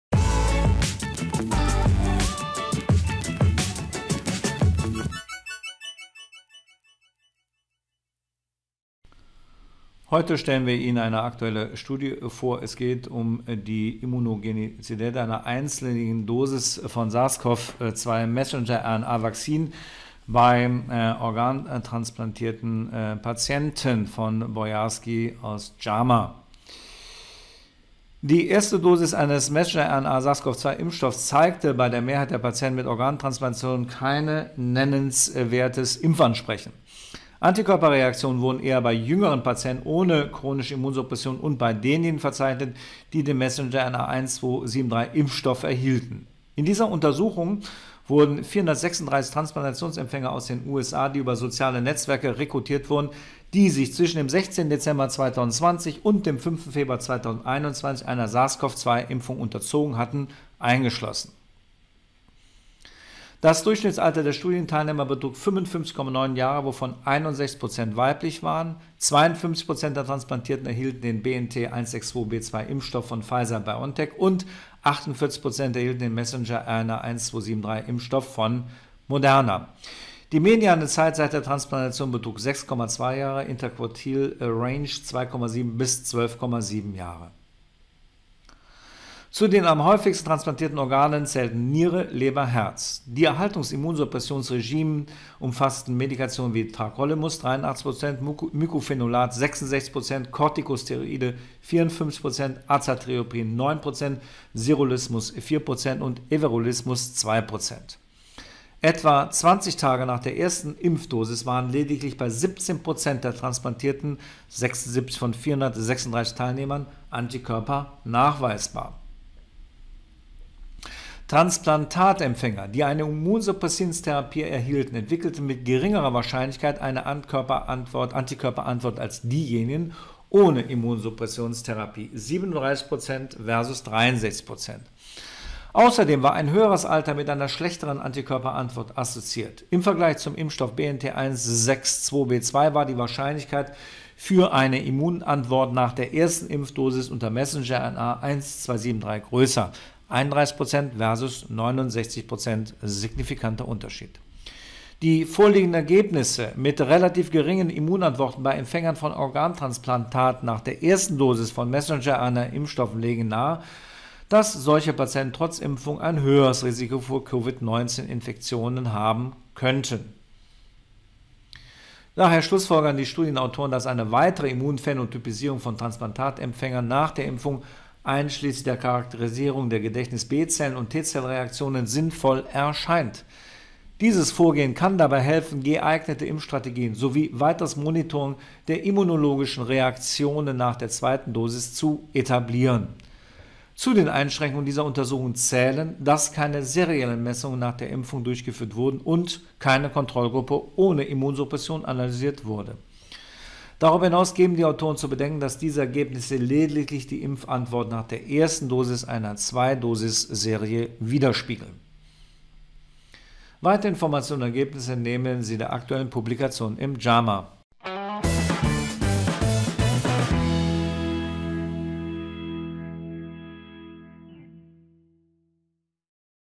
COVID-19 Studie - Impfansprechen bei Transplantierten gering
Quellen:
Deutsche Gesellschaft für Internistische Intensivmedizin und Notfallmedizin (DGIIN)